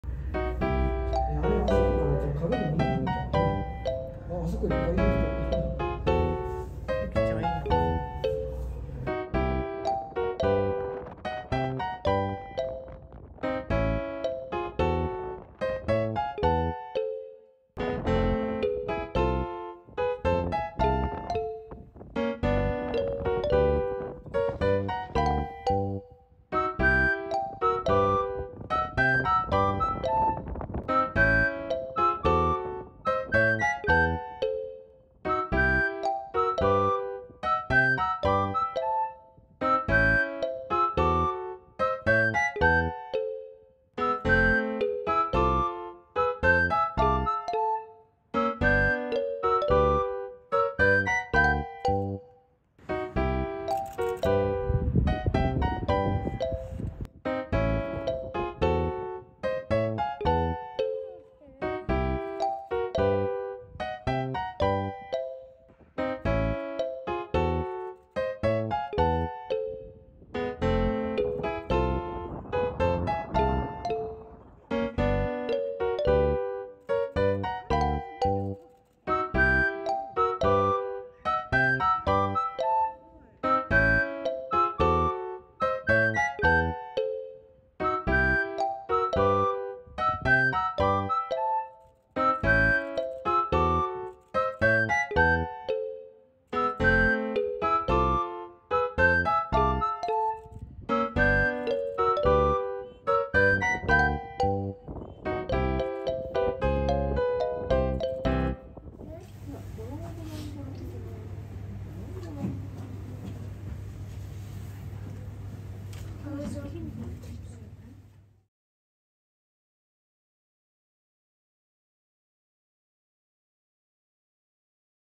【男鹿水族館GAO】ホッキョクグマにのフブキ、シロクマが茶ぐまになるまで/polar bear
2022/11/20

フブキは2023年3月に東山動植物園（名古屋）移動が決まったそうです！
フブキが幸せに暮らせますように！！！